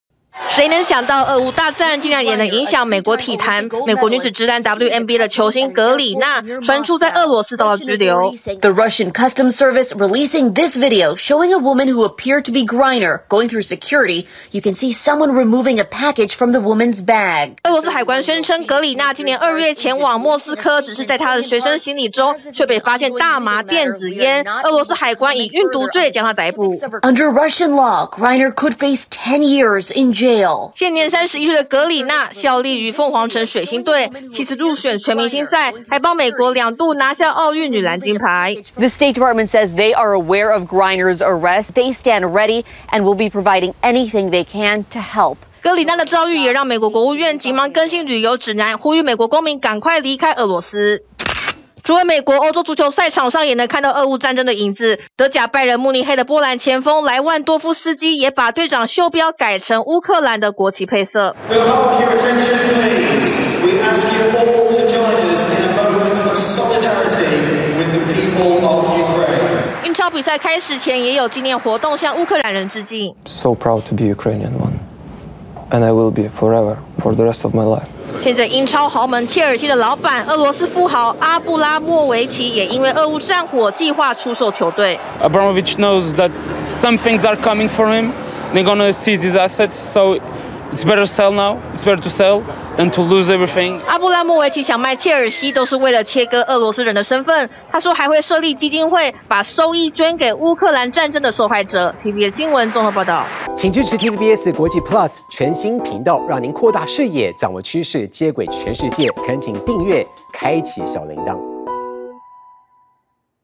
俄烏戰燒向體壇! WNBA球星遭俄拘捕 美國促在俄公民快撤｜TVBS新聞
俄烏大戰的影響，開始在體壇發酵！除了俄羅斯富豪阿布拉莫維奇，宣布賣掉英超豪門球隊切爾西外；英超聯賽也在這個週末，透過賽前紀念活動，向勇敢的烏克蘭人致敬。還有美國女子職籃WNBA的球星在俄羅斯被拘捕，也讓美國國務院緊急呼籲，在俄羅斯的公民儘速離境。